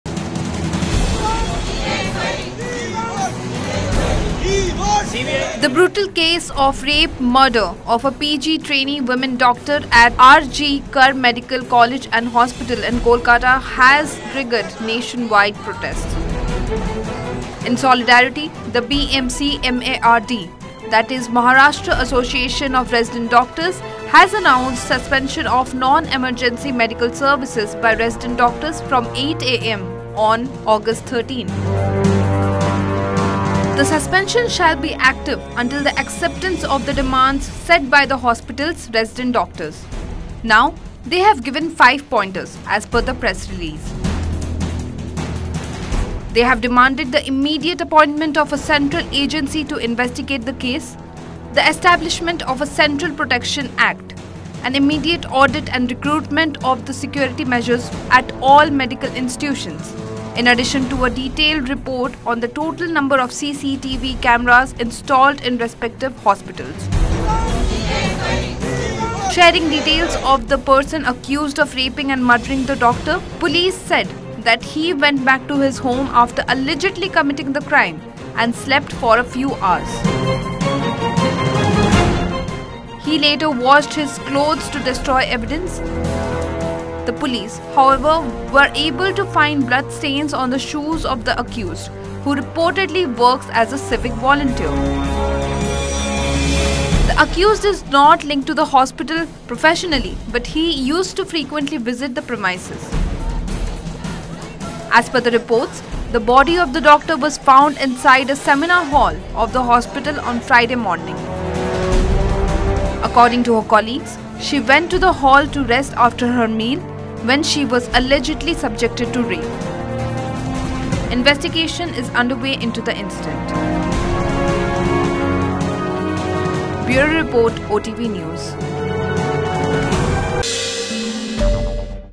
Kolkata Doctor Murder: MARD Suspends Medical Services, Police Find Vital Evidence During Probe
Murder Of Doctor In Kolkata: MARD Announces Suspension Of Medical Services, Police Find Vital Evidence